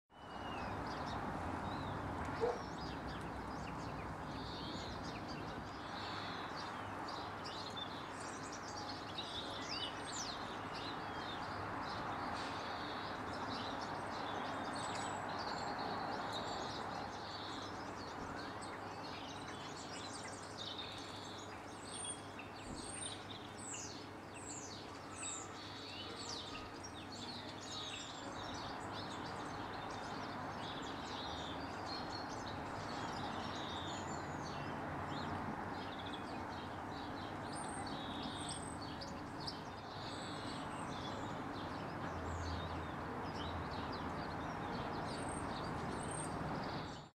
Great Spotted Woodpecker
Feeding on fat balls in garden in north west Kent, UK. 19 Feb 2013